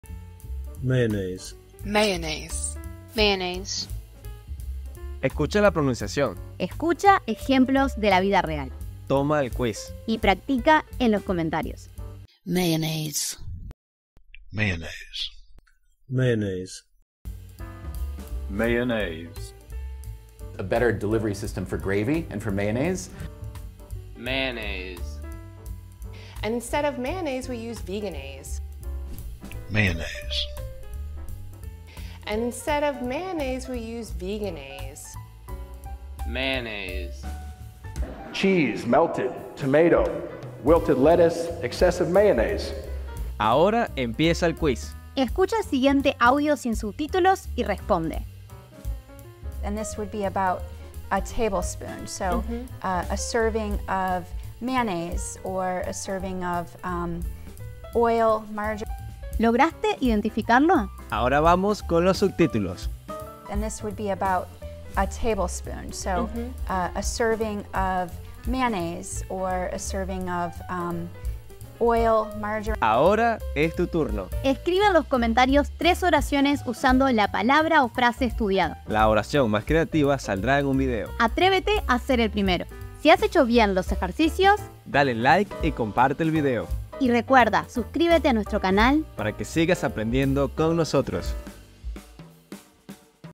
Como pronunciar: MAYONNAISE - INGLES EN ESPAÑOL / Ejemplos Reales para Hispanos
Aprende a pronunciar:  MAYONNAISE MAYO - con sonidos en Español / castellano.
👇 APRENDE INGLES EN ESPAÑOL (GUÍAS DE ESTUDIO):  👇
Gramática Básica 1: Próximamente!